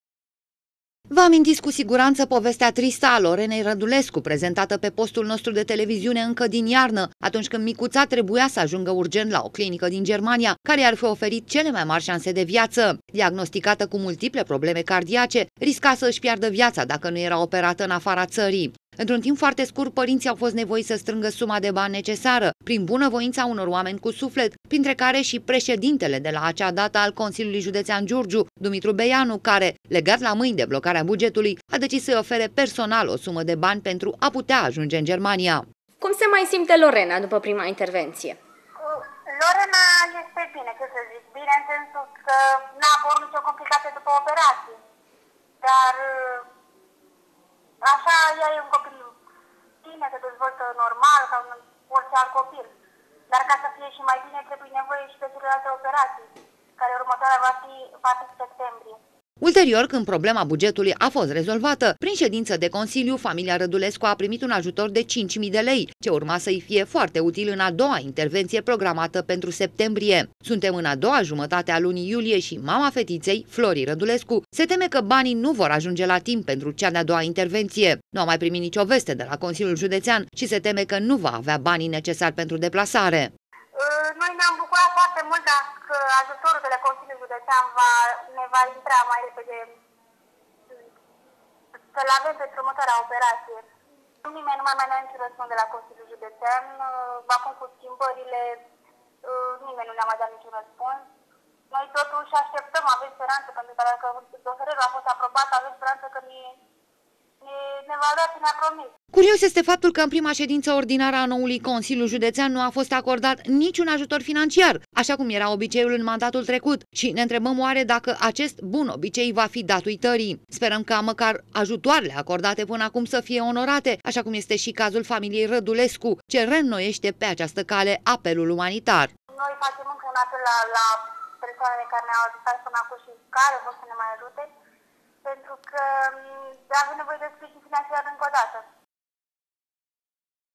IN ASTEPTAREA UNUI RASPUNS
ÎN LUNA MARTIE AJUNGEA ÎN GERMANIA PENTRU PRIMA OPERAŢIE SALVATOARE CU SPRIJINUL MAI MULTOR OAMENI DE SUFLET CARE I-AU OFERIT AJUTOR FINANCIAR. MICUŢA LORENA RĂDULESCU A TRECUT CU BINE PESTE PRIMA INTERVENŢIE ŞI SE PREGĂTEŞTE PENTRU CEA DE-A DOUA. PĂRINŢII FETIŢEI REÎNNOIESC APELUL ŞI SPERĂ CA PÂNĂ ATUNCI SĂ PRIMEASCĂ ŞI AJUTORUL FINANCIAR DE 5.000 DE LEI APROBAT PRIMĂVARA TRECUTĂ.